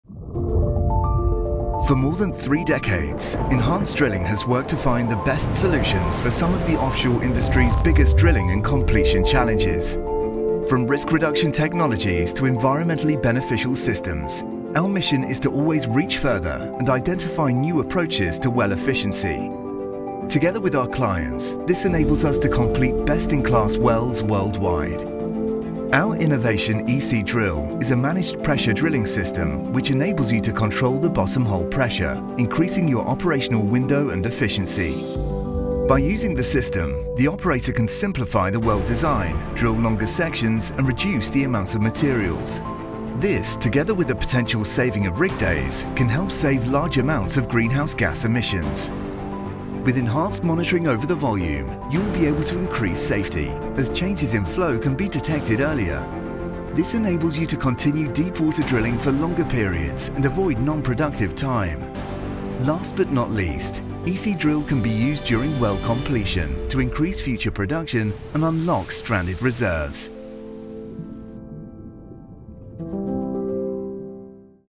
Enhanced Drilling EC-Drill®
EC-Drill® is a Controlled Mud Level (CML) system that enables operators to 'drill the undrillable' well. It solves a long-standing challenge commonly encountered in many deep-water wells: drilling within a narrow pressure window. With traditional techniques, the slightest excess or lack of wellbore pressure while operating in a narrow pressure window can mean the difference between success or failure: too much pressure and the formation will fracture; not enough will lead to hole instability and increase the chances of influx.

Enhanced Drilling provides specialist drilling technology and services to the offshore oil and gas industry.

Visit us: Enhanced-drilling.com